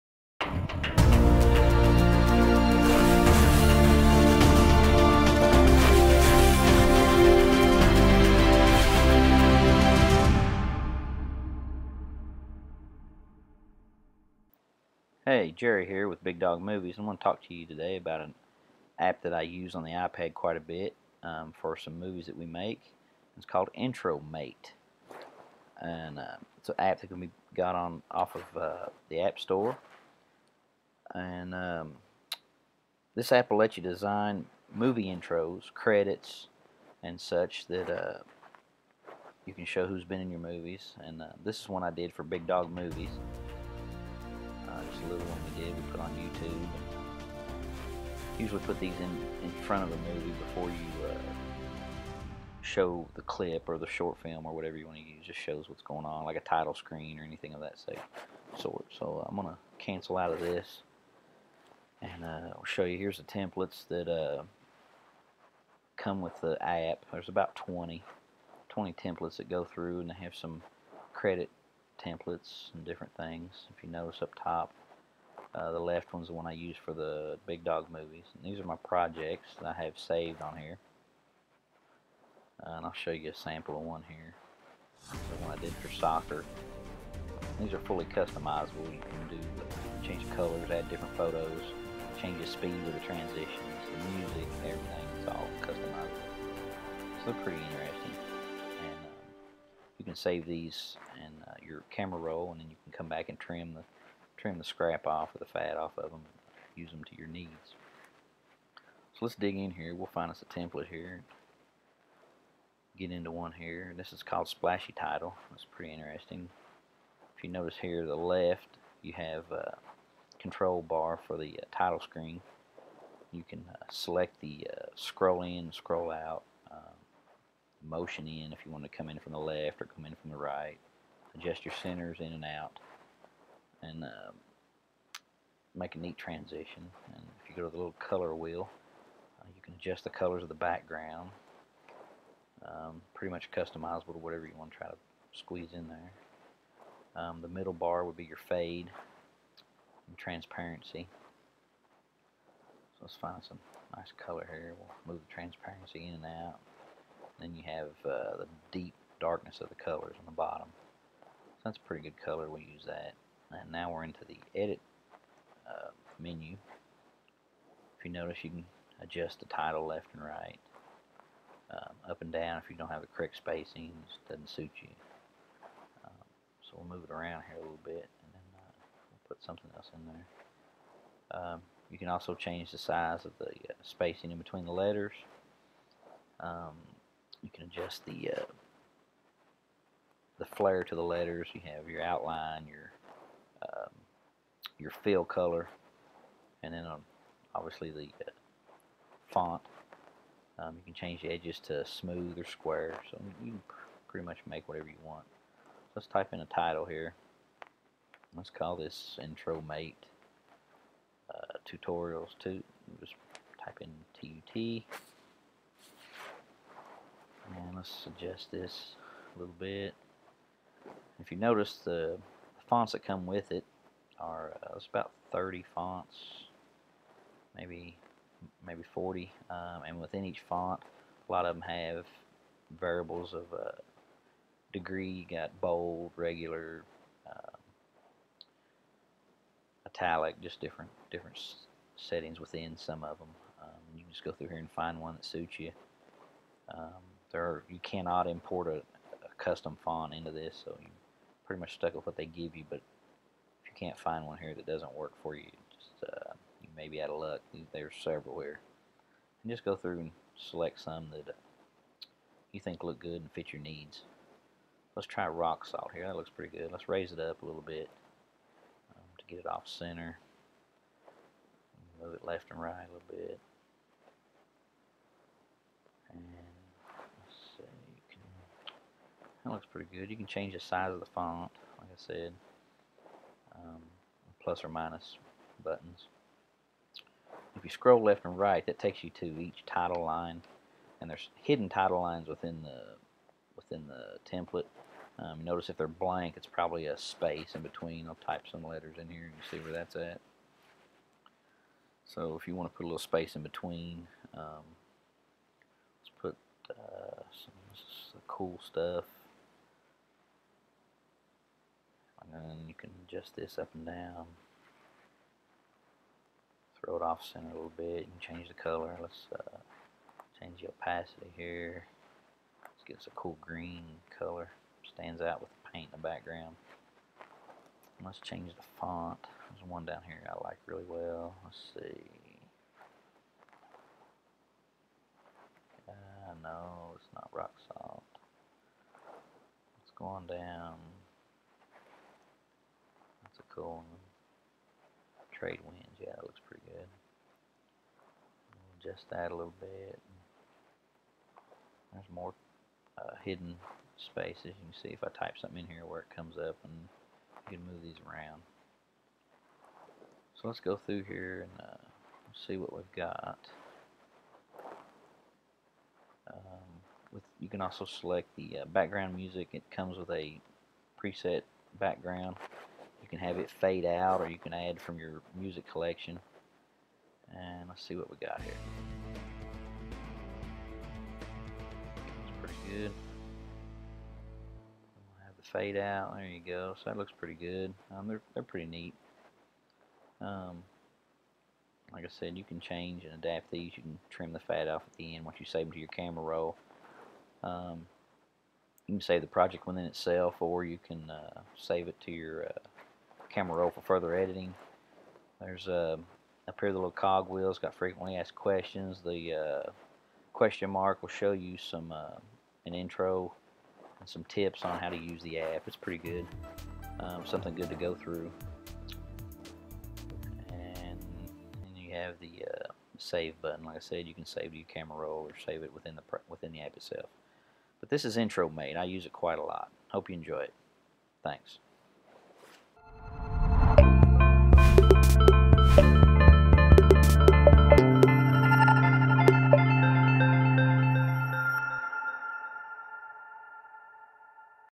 IntroMate Demo
How to use the app IntroMate on iPad